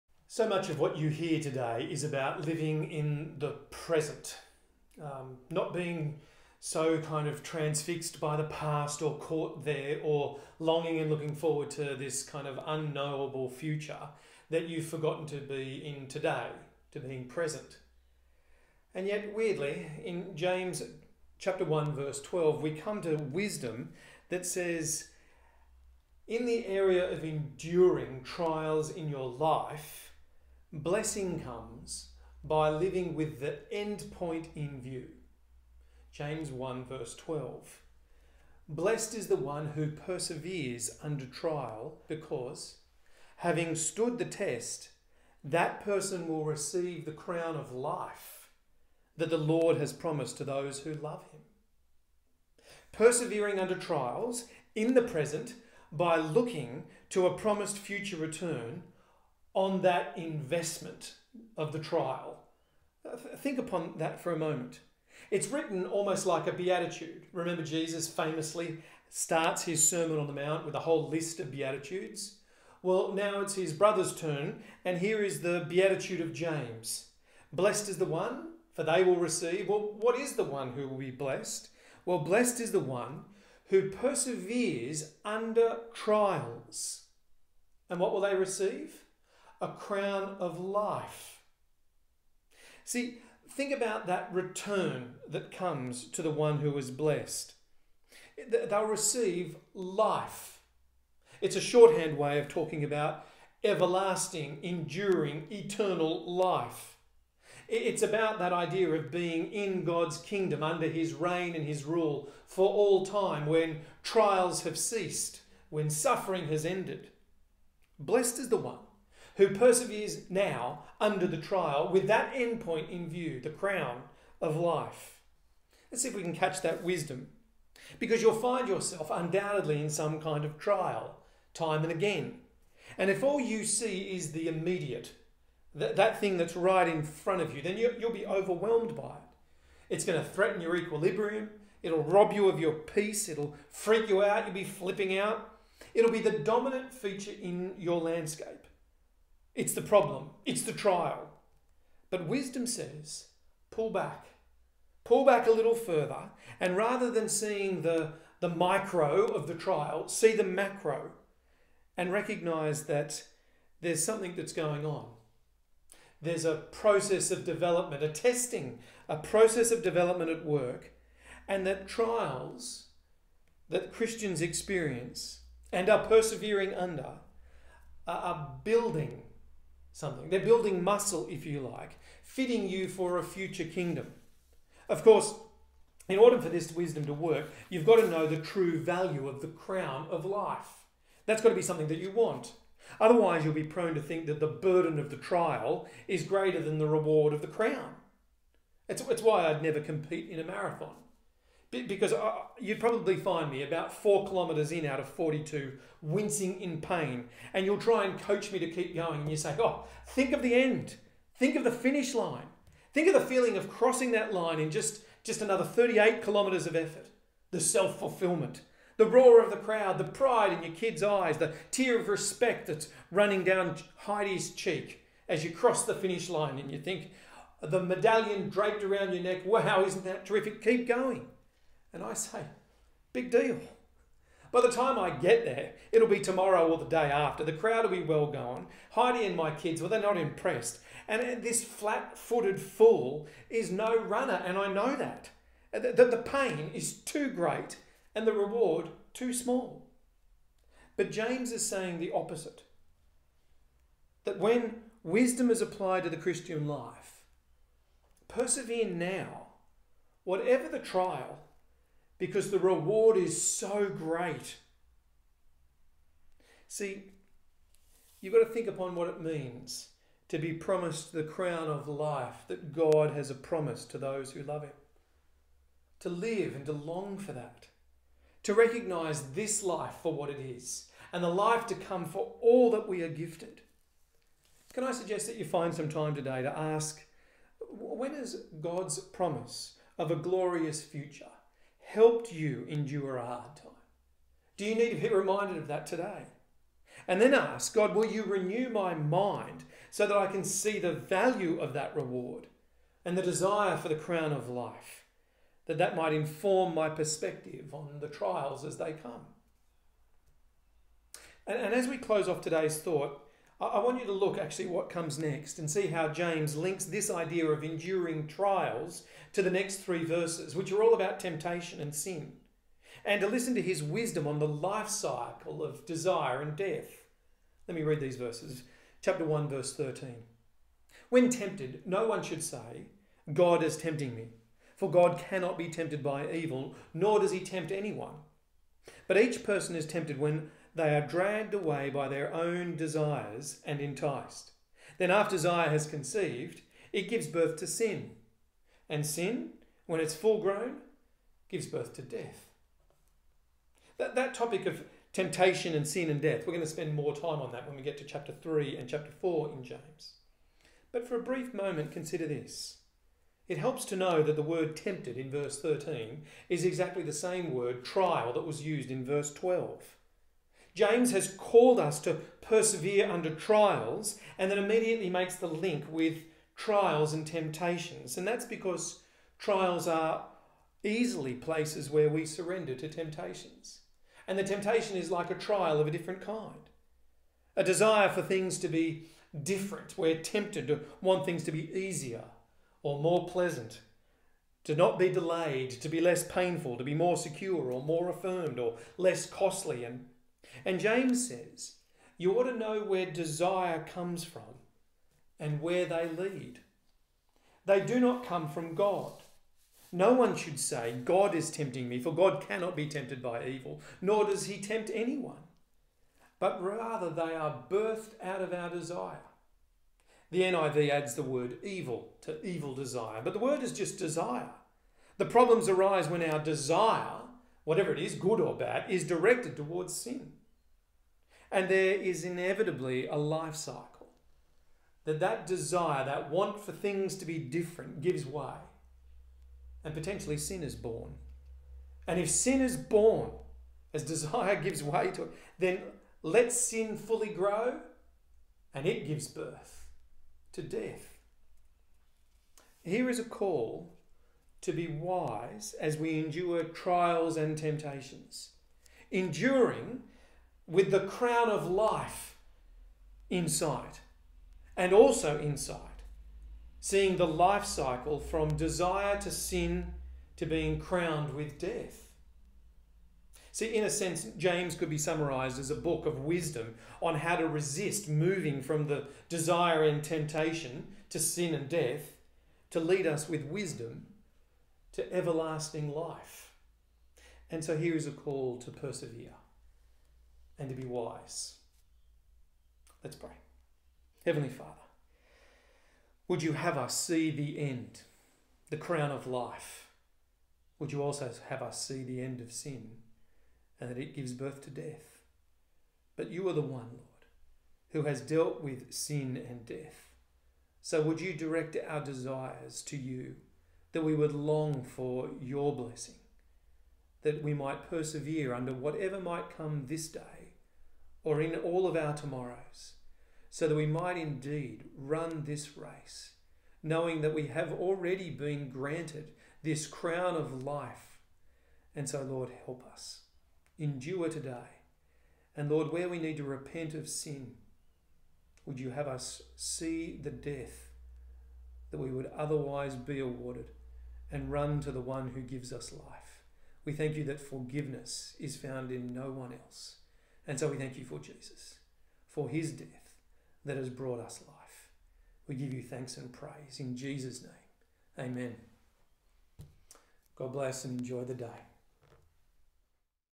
James Devotion: 1:12-15
JAMES 1:12-15
12 Blessed is the one who perseveres under trial because, having stood the test, that person will receive the crown of life that the Lord has promised to those who love him.
13 When tempted, no one should say, ‘God is tempting me.’ For God cannot be tempted by evil, nor does he tempt anyone; 14 but each person is tempted when they are dragged away by their own evil desire and enticed. 15 Then, after desire has conceived, it gives birth to sin; and sin, when it is full-grown, gives birth to death.